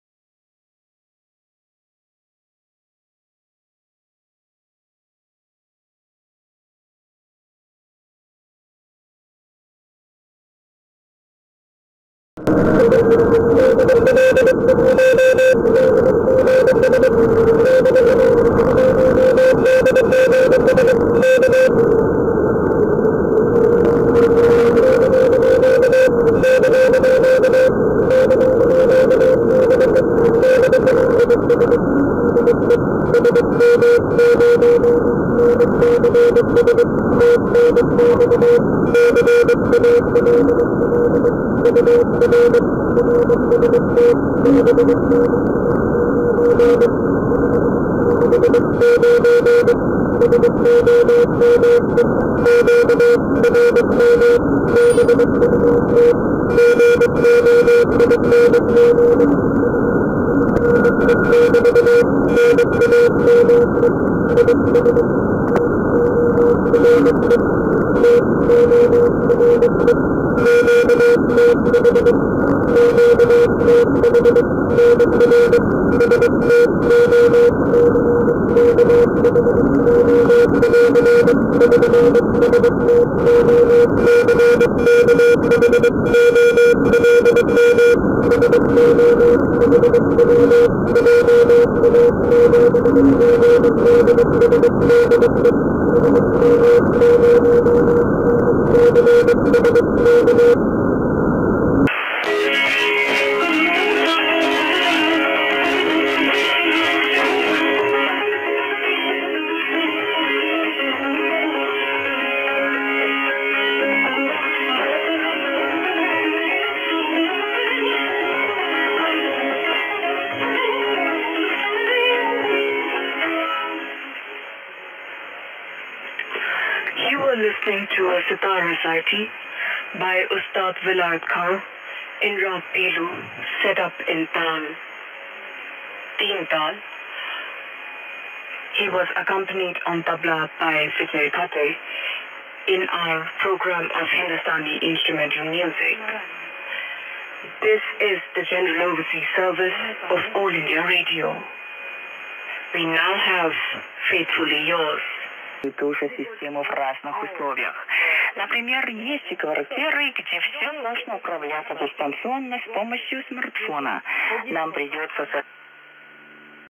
CW and AM Broadcast Band Reception using NooElec Upconverter with RTLSDR Dongle
Poor man's SDR! My first attempt to tune into the HF [High Frequency] Short Wave band [3-30 MHz] using the poor man's US$10 Software Defined Radio Dongle RTL2838U with an Upconverter from Nooelec Inc. USA. RTL2832/38U with R820T receives VHF, UHF and up to 1700 MHz. With the upconverter manufactured by NOOELEC.COM the entire window to the short wave band also opens up.